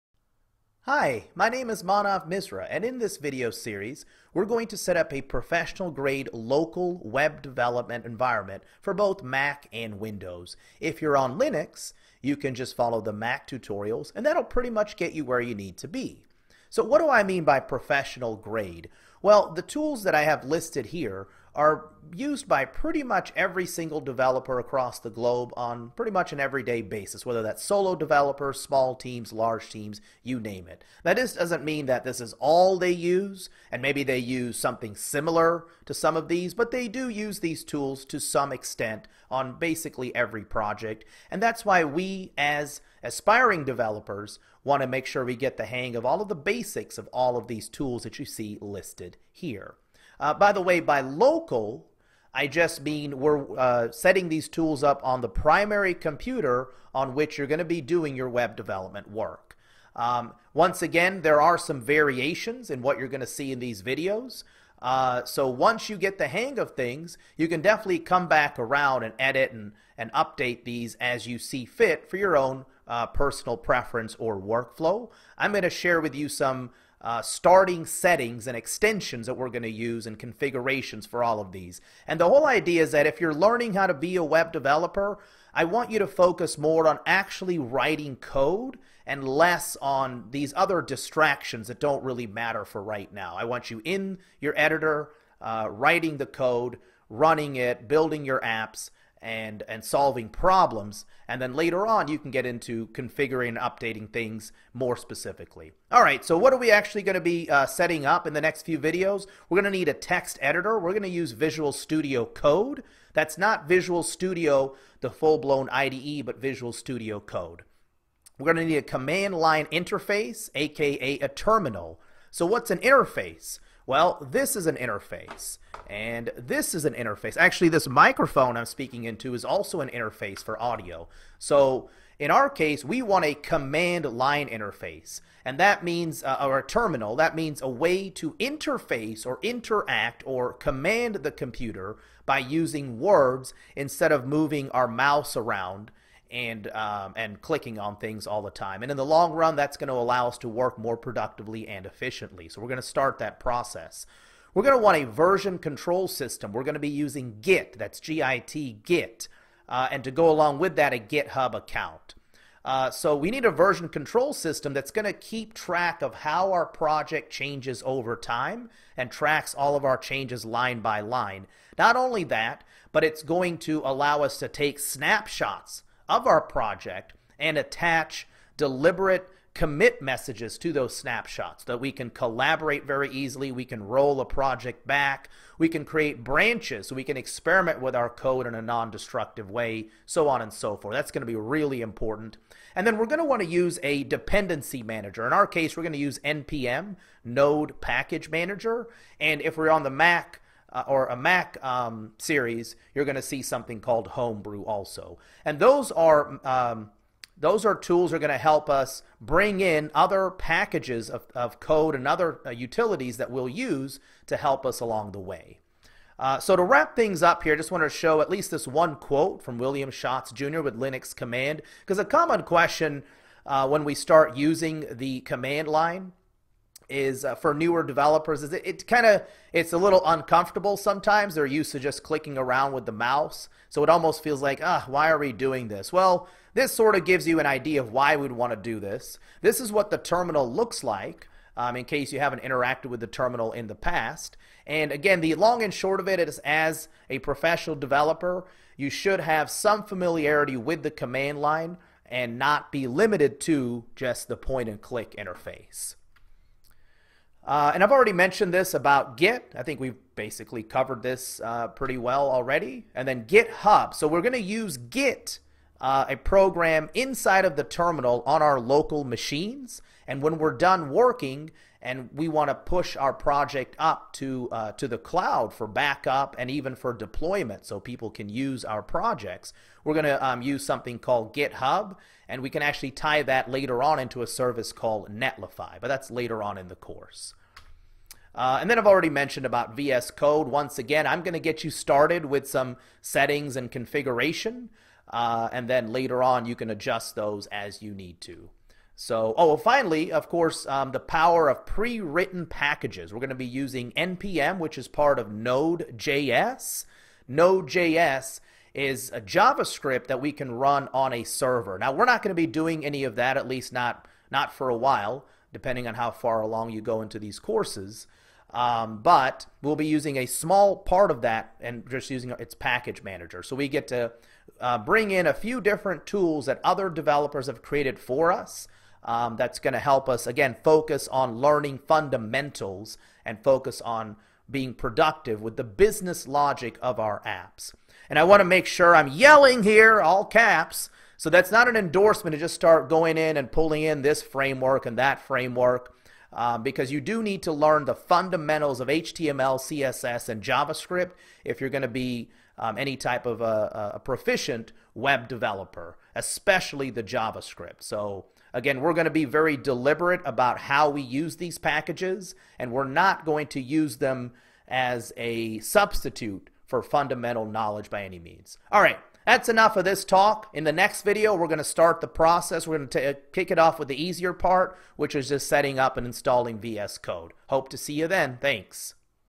Beginner's Guide to Setting Up a Professional-Grade Local Development Environment
- 3:58 - Terminal
- 4:15 - `git`/GitHub
- 4:50 - VS Code
- 5:01 - Node/`npm`

There's a lot to learn when starting your journey towards being a web developer. In this series, we kick off the process of streamlining your educational efforts by outlining the tools associated with a local developer environment and what that actually means.

Next week's release will cover the first part of Mac/Linux (Windows will be the following).

The full course covering Windows, Mac and Linux will be available on greatercommons.com soon! 🤓Meanwhile, weekly videos in this series will be released.